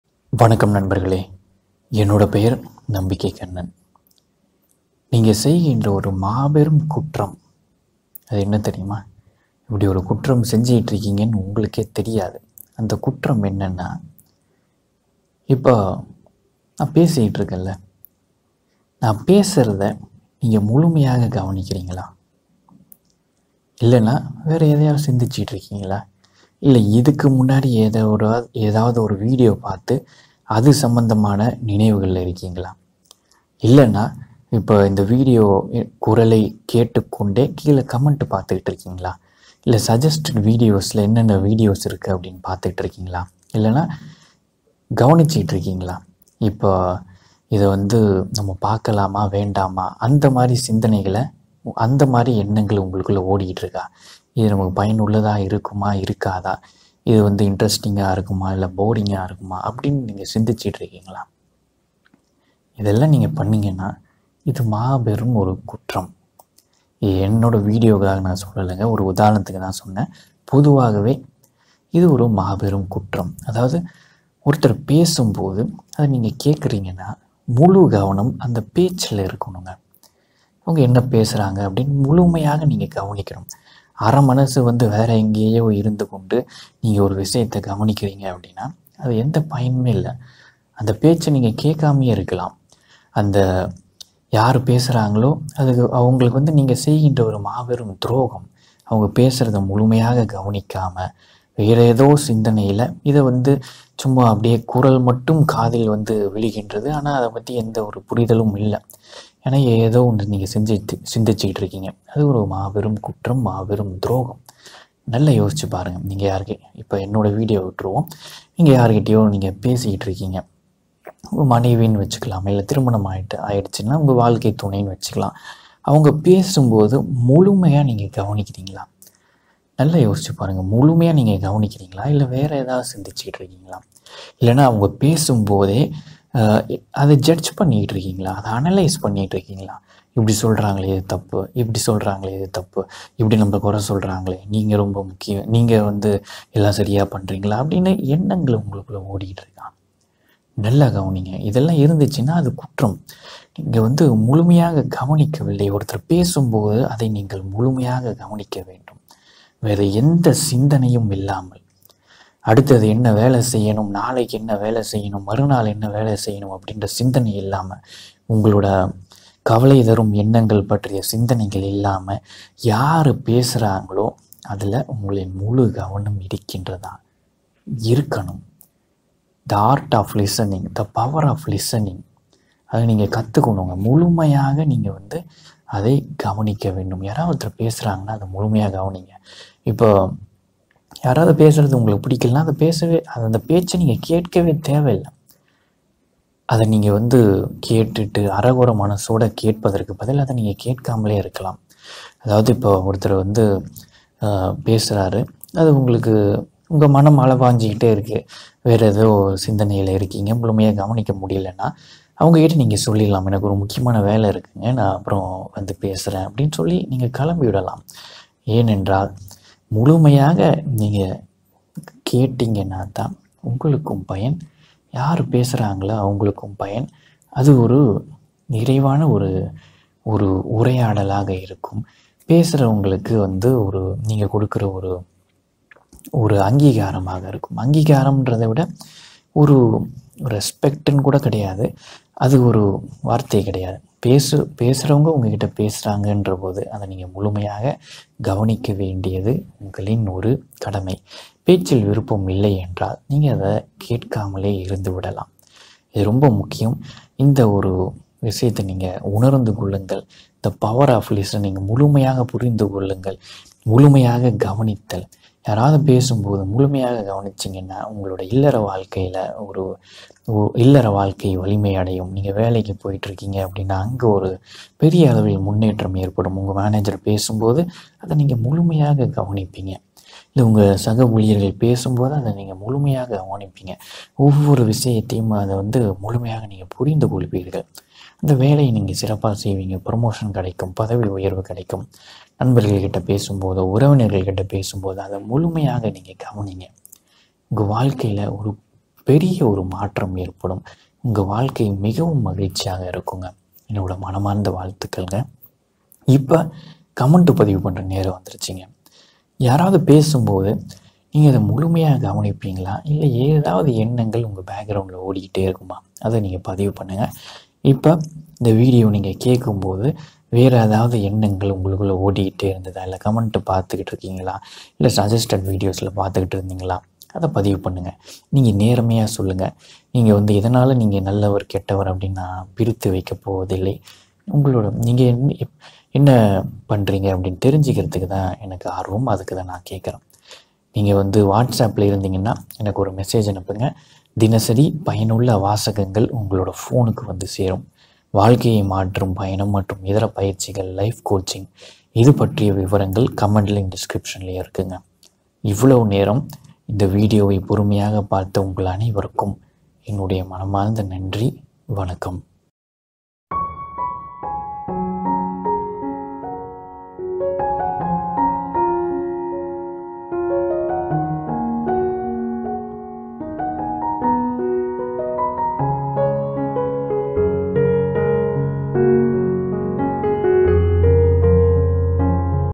Please stop this bad habit|இந்த கெட்ட பழக்கத்தை நிறுத்துங்கள்|Vaanga pesalam|Nambikkai Kannan
”நான் எந்த குற்றமும் செய்யவில்லை” என்று நீங்கள் நம்பிக் கொண்டிருந்தால், இந்த வீடியோ அது பொய் என்பதை உணர்த்தும்.

“I have not done any crime in life". If you are believing this, this video will prove that you are wrong.